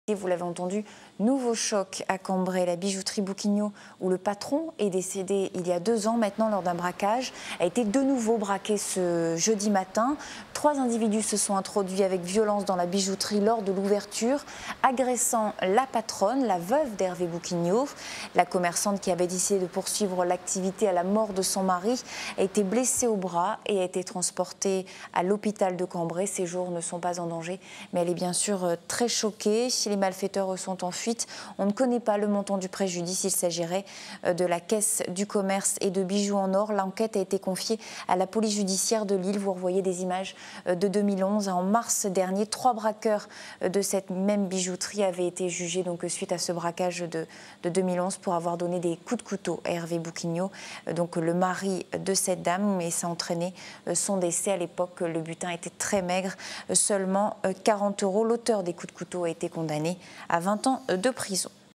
Cambrai : la bijouterie Bouquignaud à nouveau braquée
Nouveau choc à Cambrai. La bijouterie Bouquignaud où le patron est décédé il y a deux ans lors d'un braquage a été de nouveau braquée ce jeudi. Trois individus se sont introduits avec violence dans le magasin lors de l'ouverture agressant la patronne, la veuve d'Hervé Bouquignaud. Les malfaiteurs sont toujours en suite. On ne connaît pas pour le moment le montant du préjudice.